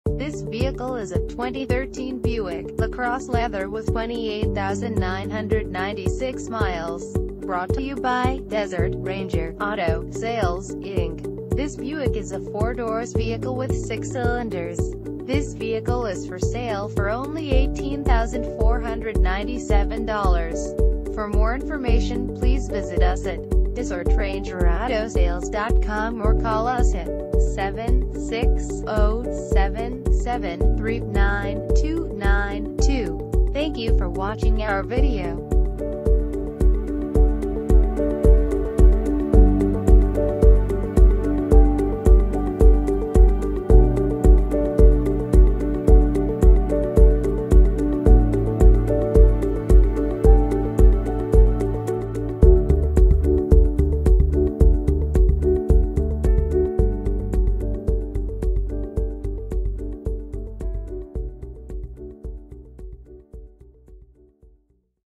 2013 Buick LaCrosse Leather - Desert Ranger Auto Sales Inc.
Desert Ranger Auto Sales Inc.
74804 Joni Dr. Suite 1a

One owner vehicle, Clean title, Manufacturer Warranty, 28,996 miles, Automatic, Mocha Bronze Metallic exterior, Cashmere interior, Sedan, FWD: Front Wheel Drive, 4 doors, 6 cylinders, Gasoline, MPG City 17 and 27 in Hwy.